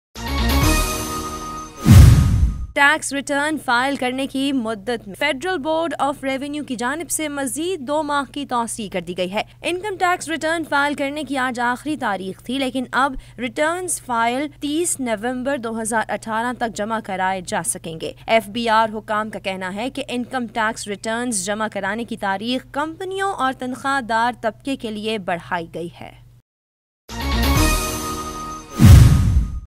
Federal Board of Revenue extended more than 2 months in the tax return file period
Federal Board of Revenue extended more than 2 months in the tax
return file period